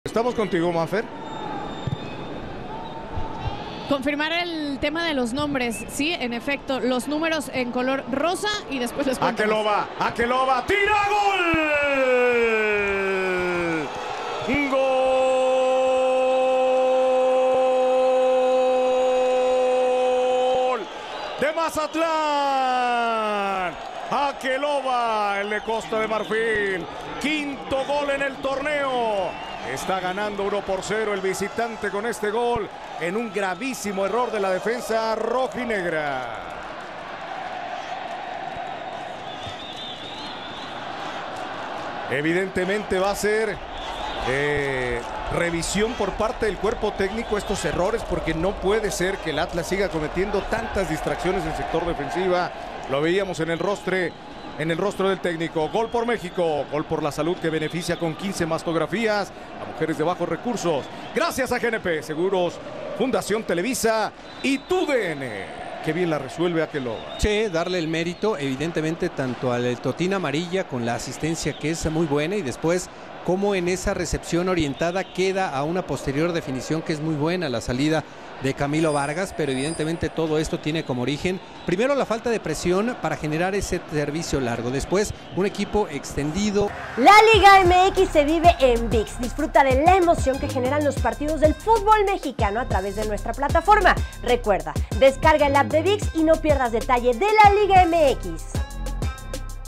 Gol de Aké Loba | Atlas 0-1 Mazatlán | Liga BBVA MX | Apertura 2023 - Jornada 13
¡¡¡G⚽⚽⚽L!!!

¡¡Aké Loba anotó el primer gol del partido!! Los Cañoneros ya vencen a los Rojinegros.